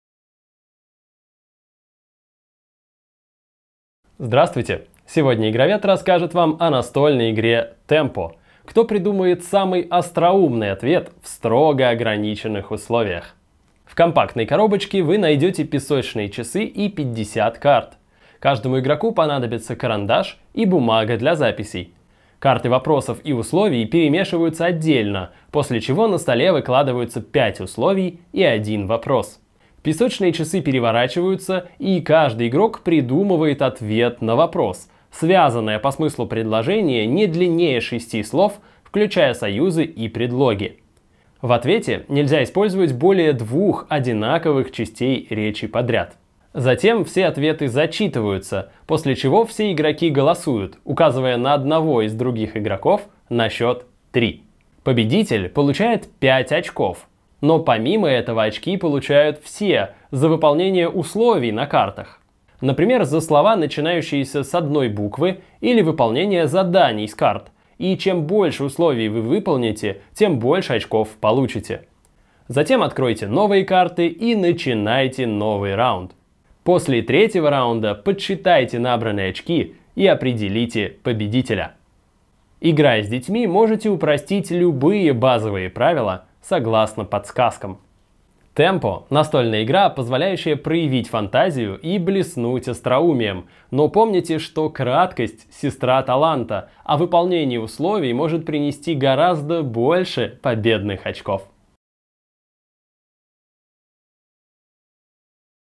Темпо. Обзор настольной игры от Игроведа
Зажигательный пати-гейм, где надо отвечать на странные вопросы, соблюдая условия.

Возраст 12+
Время игры 15-30 минут
Количество игроков 2-10

Обзор настольной игры Темпо от Игроведа.
Проявите смекалку и фантазию и предложите остроумные ответы на самые разнообразные вопросы! «Проще простого!» – скажете вы. А что если вам нужно дать ответ, соблюдая определённые условия? И у вас всего одна минута? Скорее засекайте время и узнайте, кто сумеет ответить на вопросы в заданном темпе!

И приходите к нам в магазины Игровед в Москве и Санкт-Петербурге.